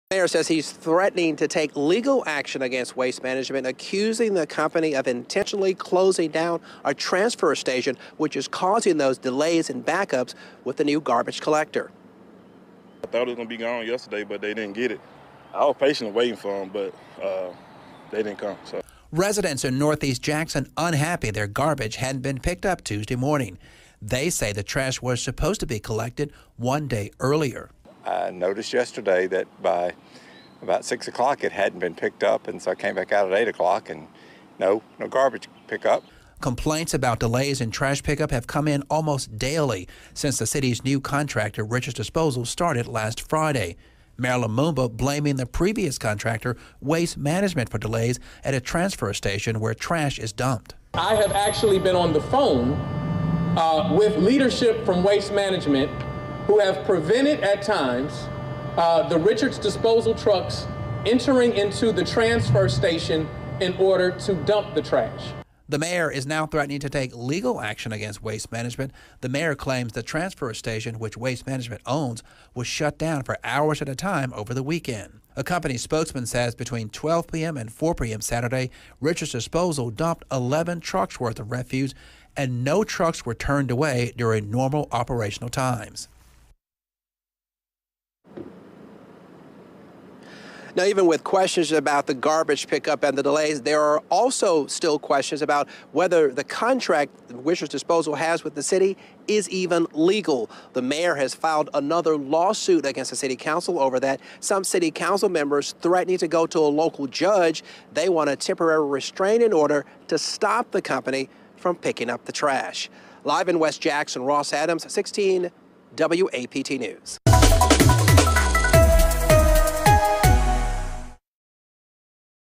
Waste Management V. Jackson Mayor
Mayor threatens legal action against Waste Management